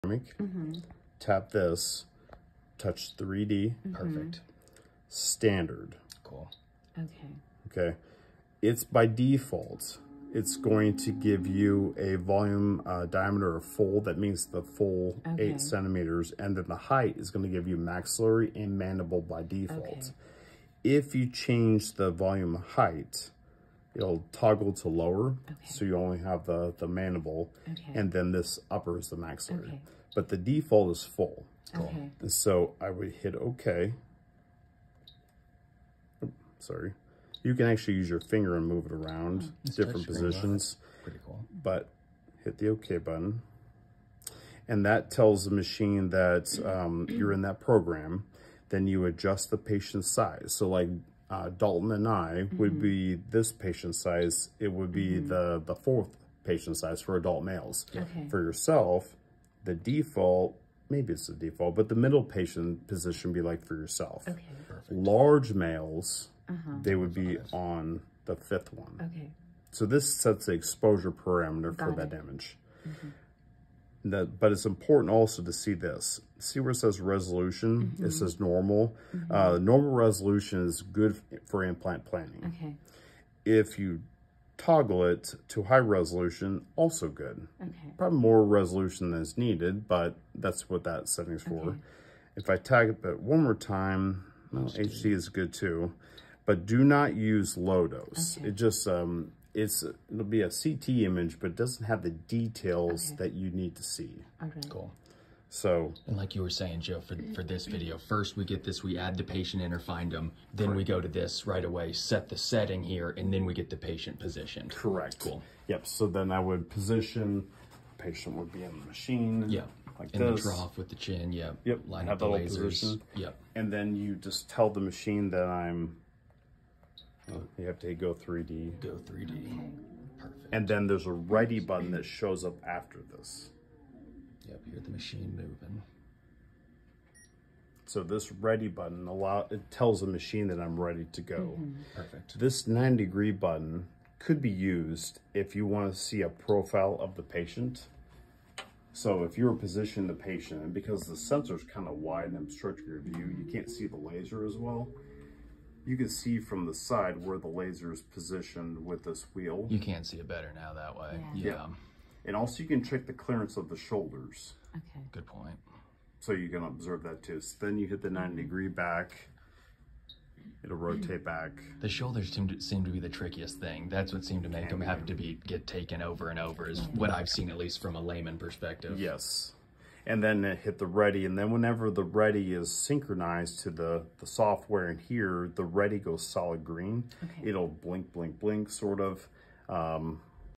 Cone Beam Settings | Planmeca Imaging
Here are the Cone Beam configurations for the Planmeca ProMax Imaging System. As you can see, this is state of the art dental tech!!